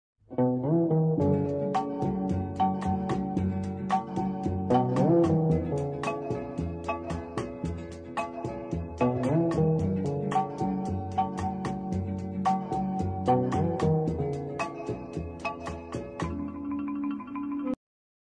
HAND PAINTED ABSTRACT FLORALS | TUTORIAL PREVIEW | FURNITURE PAINTING | UPCYCLE
Tutorial preview! Learn how to hand paint soft abstract florals with @DixieBellePaint Terra Clay Paint!